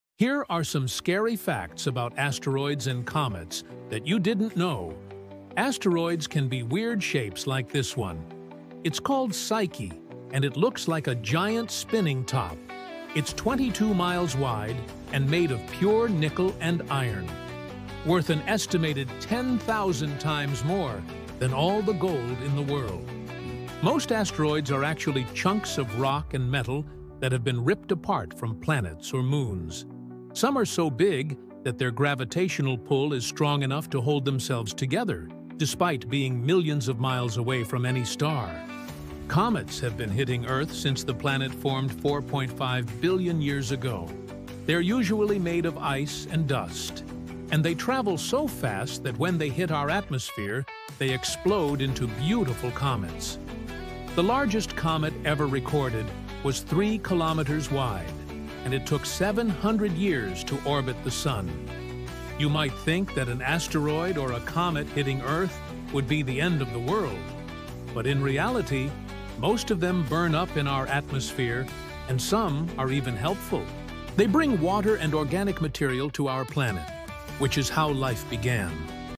Weird facts about asteroids and comets
Asteroids and comets are full of surprises! Some comets can develop tails millions of miles long, made of gas and dust blown by the sun’s heat. The asteroid belt isn’t as crowded as movies show—spaces between rocks can be huge! And some asteroids are actually “rubble piles,” loose collections of rocks held together by gravity.